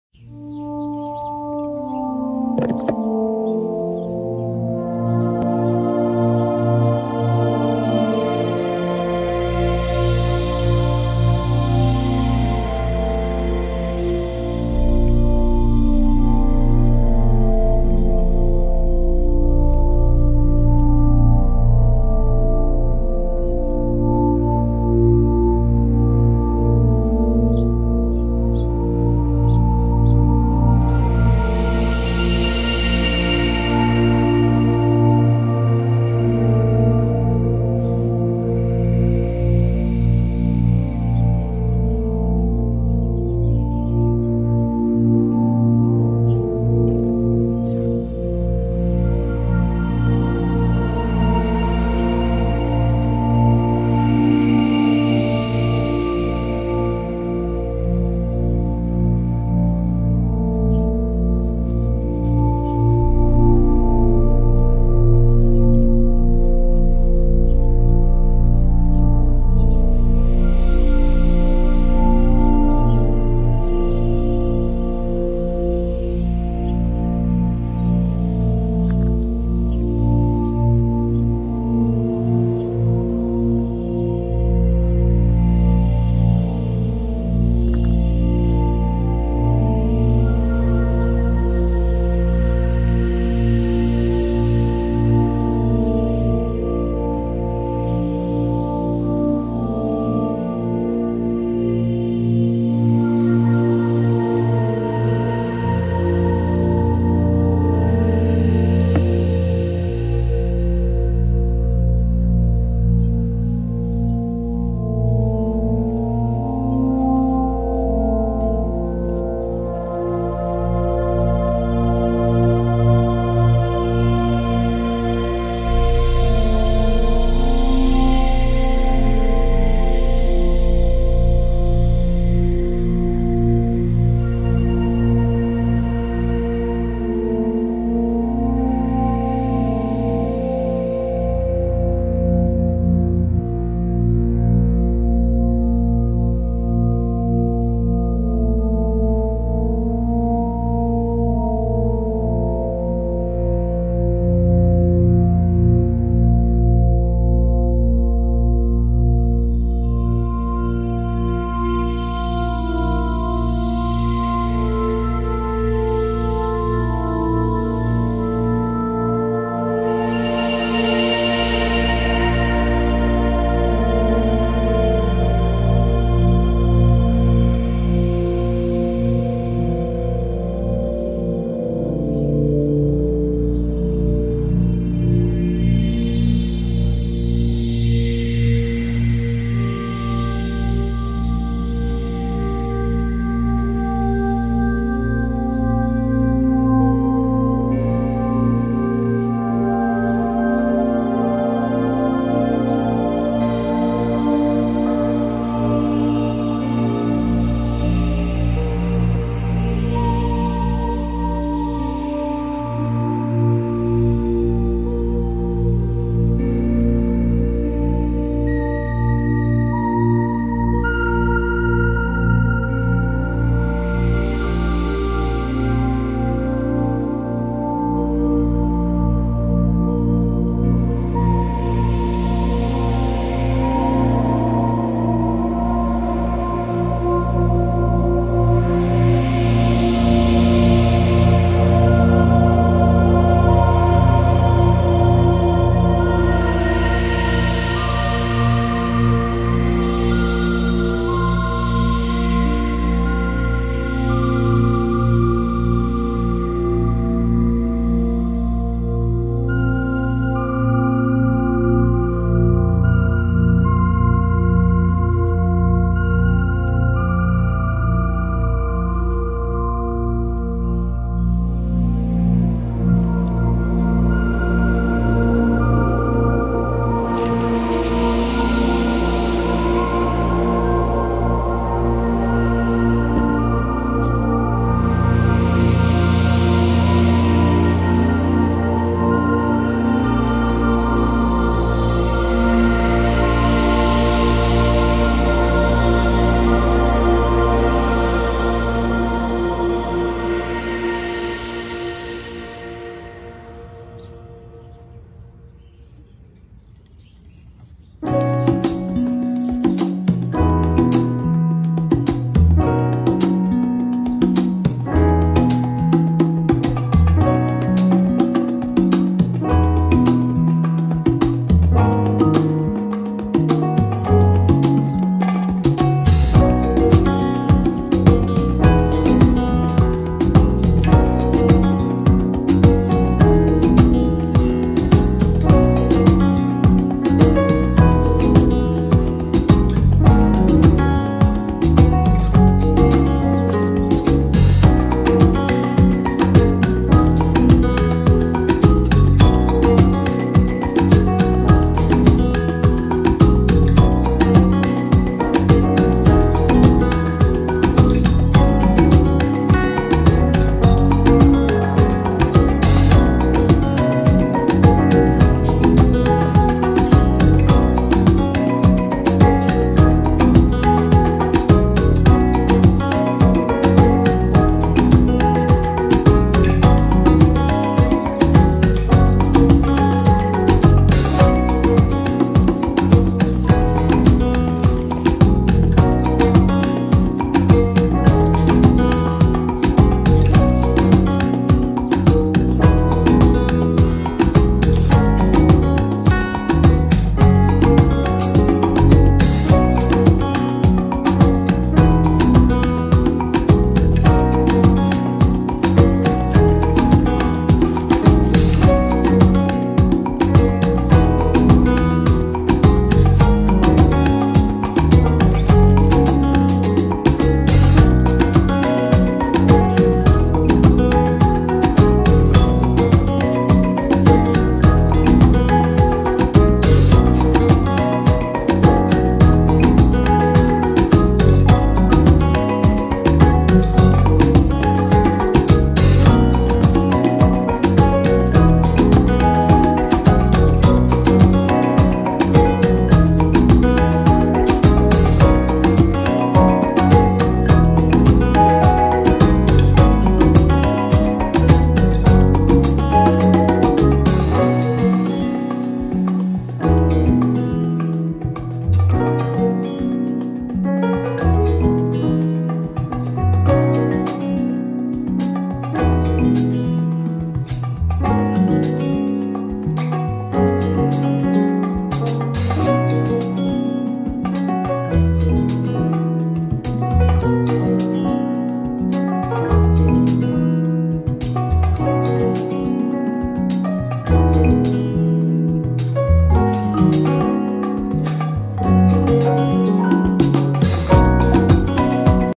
CORLORFUL HANGING METAL SUCCUELNT ARRAGEMENT // RUBY NECKLESS // SUPERBUM // PORK AND BEANS // KIWI
Hi friends, In today's video I have a fun and colorful hanging arrangement to share. This metal arrangement was recently spray painted and I had so much fun planting it up. I started playing with more colorful pots in my container garden. I love the pops of color and the life it brings to the outdoors. I used only a few succulents in these four containers. The new plants I got for this arrangement were the Ruby necklace, and superbum. I also used Aeonium "kiwi", pork and beans, string of tears, and mikes fuzzy wuzzy.